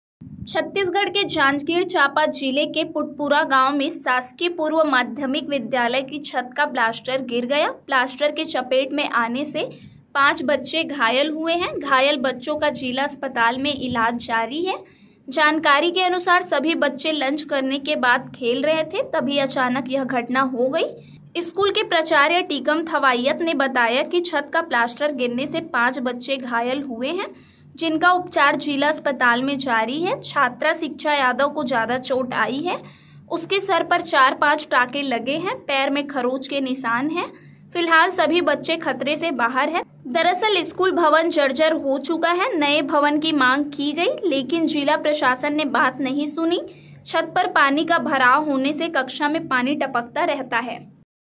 CG News : जांजगीर-चांपा ग्राम पुटपुरा के शा. पूर्व मा. विद्यालय की छत का प्लास्टर गिरा 5 बच्चे घायल
CG News: Sha of Janjgir-Champa village Putpura. Former Hon. Plaster of school ceiling fell, 5 children injured